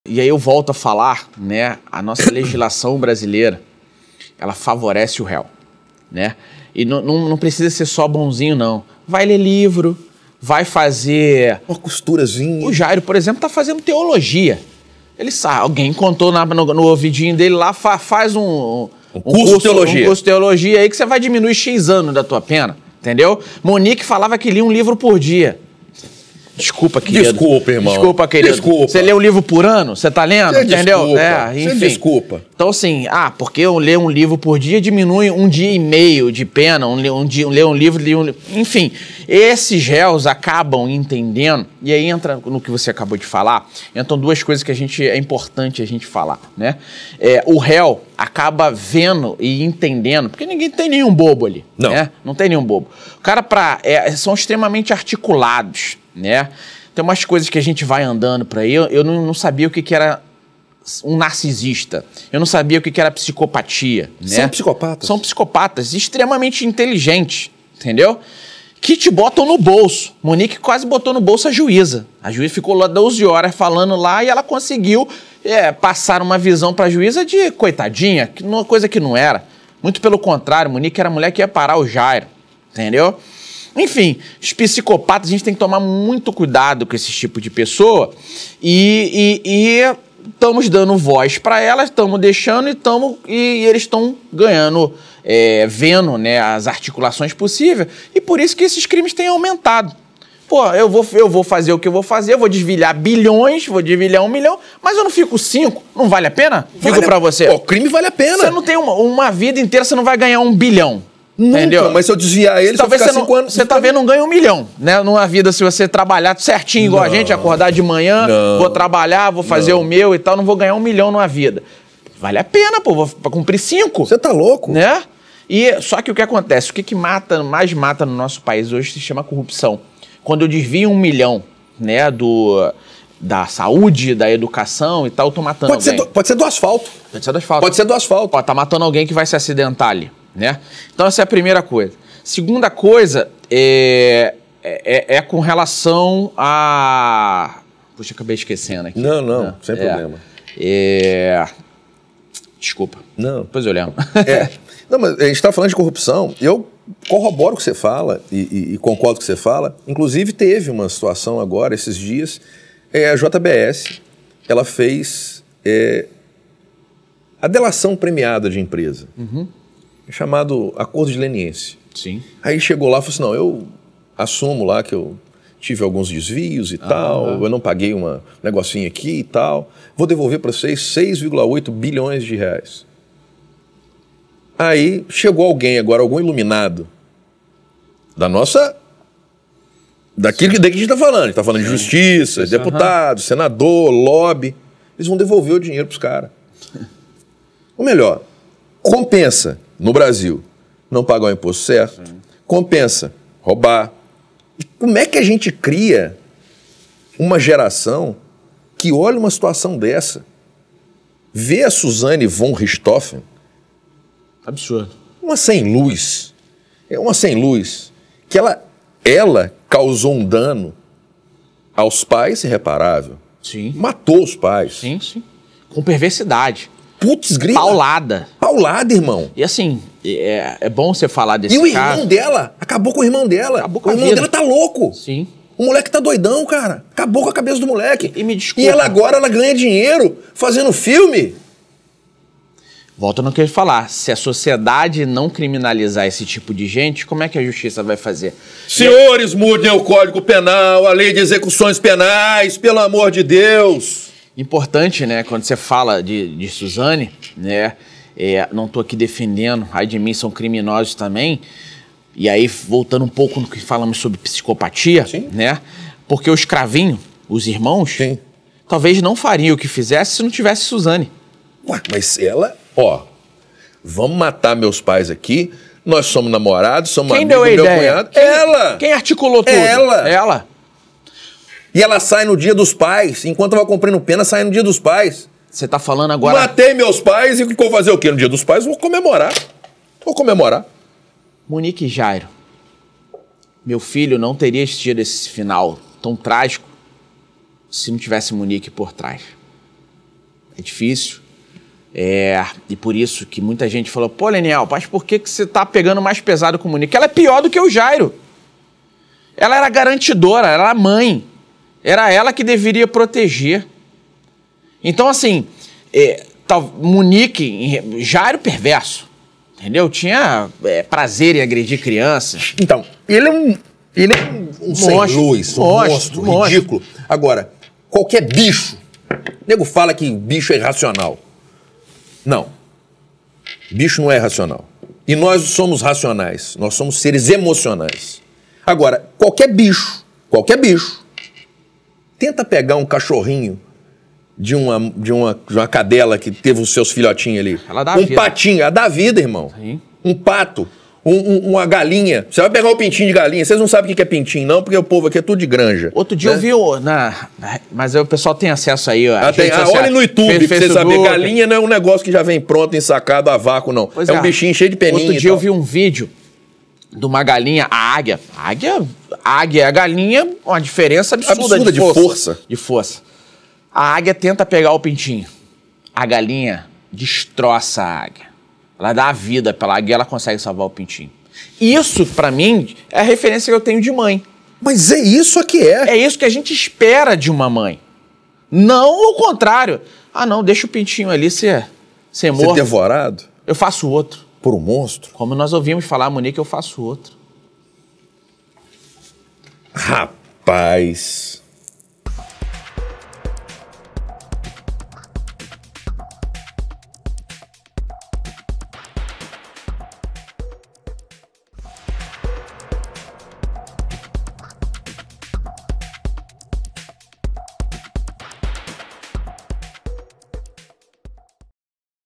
LENIEL BOREL APONTA OS RISCOS DE SE CONVIVER COM NARCISISTAS
A convivência com narcisista pode ser um risco ainda maior sob o regime de um Estado que conta com uma legislação que não oferece penas severas a quem não faz questão de seguir dentro da lei, aponta Leniel Borel